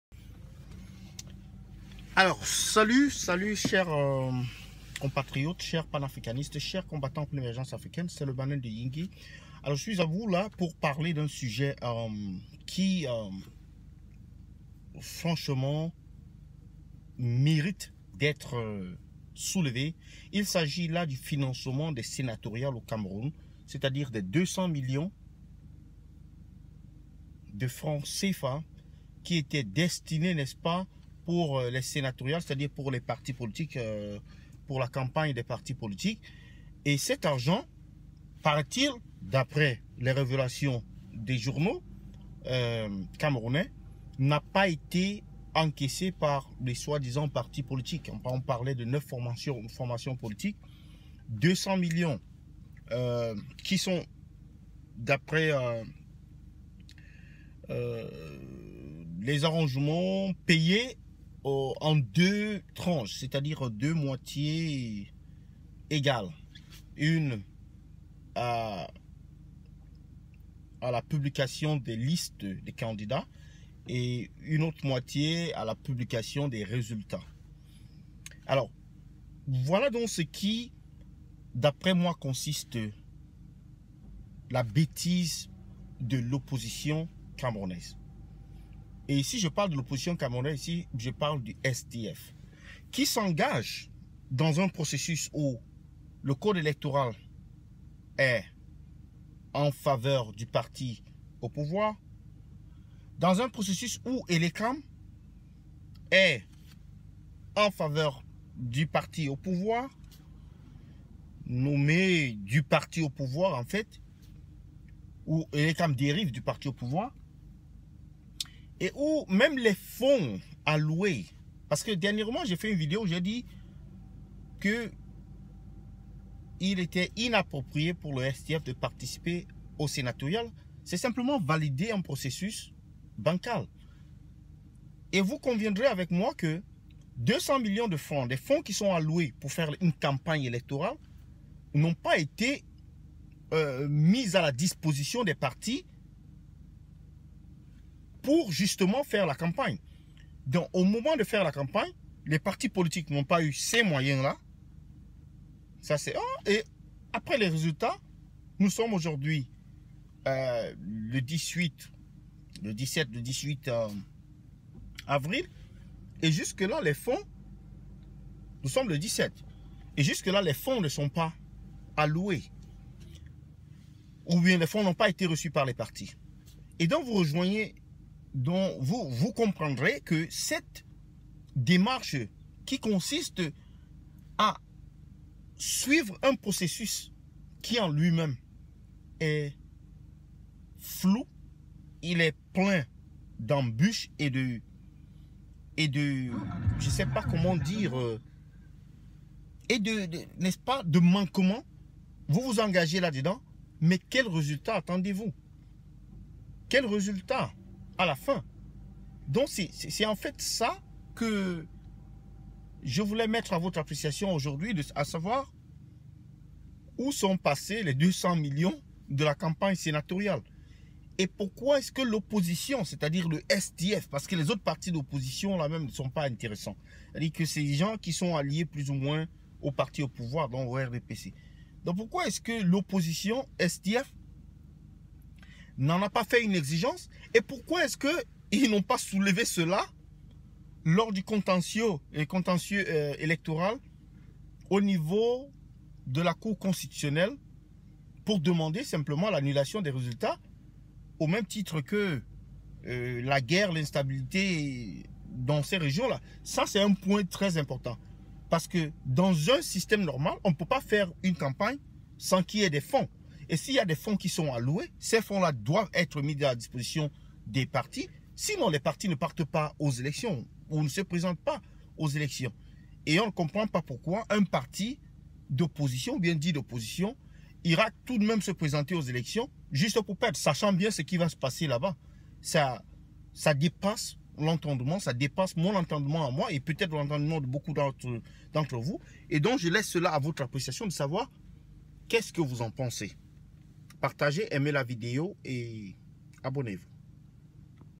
OÙ SONT PASSÉS LES 200 MILLIONS DE FRANCS CFA DESTINÉS AUX PARTIS POLITIQUES POUR LA CAMPAGNE DES S
Où sont passés les 200millions alloués à la campagne des sénatoriales au Cameroun de 2018?

En effet, l’article 275 du Code électoral souligne que les partis politiques qui concourent à l’expression du suffrage, peuvent bénéficier du financement public. Et l’article 276 de préciser que ce financement concerne les dépenses couvrant les activités permanentes des partis politiques. Mais aussi celles consacrées à l’organisation des campagnes électorales, en mettant l’accent sur la répartition.

Le Code électoral explique également que les partis politiques ou les candidats bénéficiaires d’un financement public ont l’obligation de tenir une comptabilité y afférente. Ainsi, l’organisation, la composition, les attributions et les modalités de fonctionnement de la commission relèvent d’un décret présidentiel.

L’article 285 a prévu des dispositions concernant les élections des députés, des sénateurs, des conseillers régionaux ou des conseillers municipaux. Ce texte stipule que la répartition des fonds publics destinés au financement des campagnes électorales se fait en deux tranches d’égal montant aux partis en lice.

D’un côté, la première tranche. Celle-ci s’obtient, après la publication des listes de candidats. Cela s’applique au prorata, en fonction des listes acceptées dans les différentes circonscriptions électorales. De l’autre, la seconde tranche. Cette dernière est décaissée, après la proclamation des résultats, aux partis proportionnellement au nombre de sièges obtenus.

En rappel, la répartition s’applique de manière proportionnelle au nombre de sièges respectifs. Les sénatoriales se dérouleront le dimanche 25 mars prochain. Sont en course: le RDPC, le SDF, l’ANDP, l’UNDP, l’UPC, le FSNC, l’UMS, l’ANDP, l’UDC.